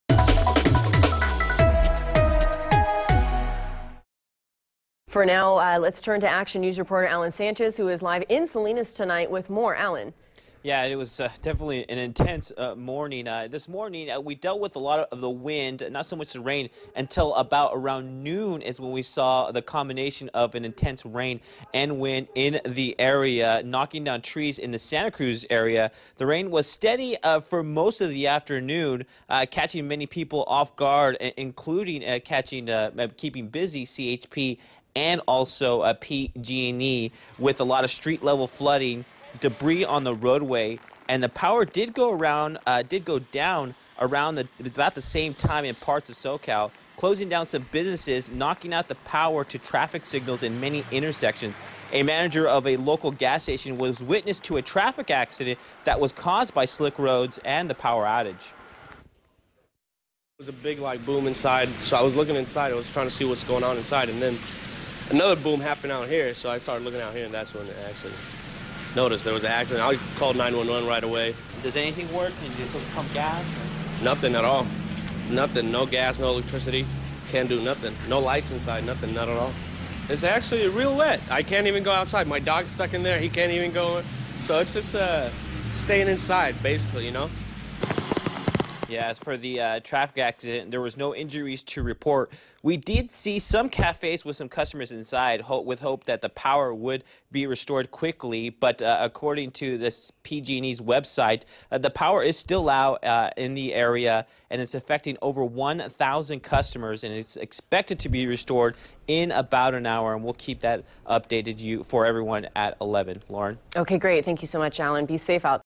Intense rain and wind take out power for parts of Soquel
Sunday's California storm kept CHP and PG&E busy with street-level flooding, debris on the roadway and power outages.